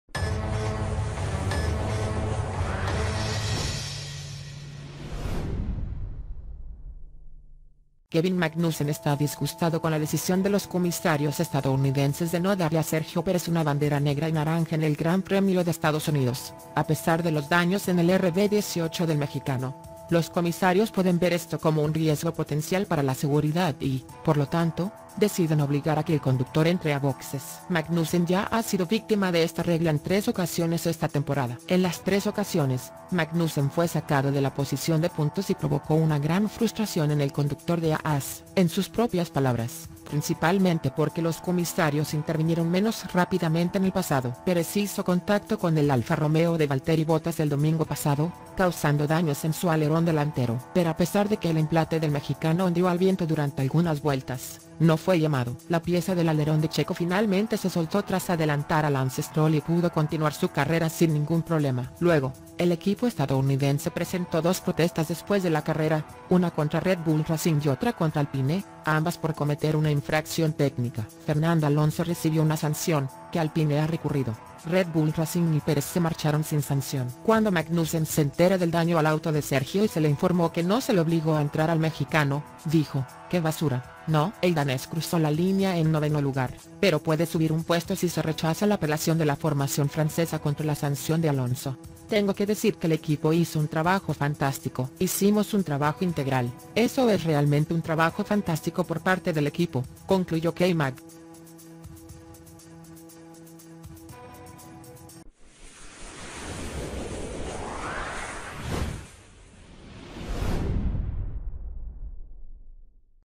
Qué basura! Magnussen enojado ve a Checo Pérez escapando del castigo en GP de Austin
Kevin Magnussen está disgustado con la decisión de los comisarios estadounidenses de no darle a Sergio Pérez una bandera negra y naranja en el Gran Premio de Estados Unidos, a pesar de los daños en el RB18 del mexicano.

Los comisarios pueden ver esto como un riesgo potencial para la seguridad y, por lo tanto, deciden obligar a que el conductor entre a boxes. Magnussen ya ha sido 'víctima' de esta regla en tres ocasiones esta temporada.

En las tres ocasiones, Magnussen fue sacado de la posición de puntos y provocó una gran frustración en el conductor de Haas. En sus propias palabras, principalmente porque los comisarios intervinieron menos rápidamente en el pasado.

Pérez hizo contacto con el Alfa Romeo de Valtteri Bottas el domingo pasado, causando daños en su alerón delantero. Pero a pesar de que el endplate del mexicano ondeó al viento durante algunas vueltas, no fue llamado.

La pieza del alerón de Checo finalmente se soltó tras adelantar a Lance Stroll y pudo continuar su carrera sin ningún problema.

Luego, el equipo estadounidense presentó dos protestas después de la carrera: una contra Red Bull Racing y otra contra Alpine, ambas por cometer una infracción técnica. Fernando Alonso recibió una sanción (que Alpine ha recurrido), Red Bull Racing y Pérez se marcharon sin sanción.

Cuando Magnussen se entera del daño al auto de Sergio y se le informó que no se le obligó a entrar al mexicano, dijo: "Qué basura, ¿no?" El danés cruzó la línea en noveno lugar, pero puede subir un puesto si se rechaza la apelación de la formación francesa contra la sanción de Alonso.

"Tengo que decir que el equipo hizo un trabajo fantástico. Hicimos un trabajo integral, eso es realmente un trabajo fantástico por parte del equipo", concluyó K-Mag.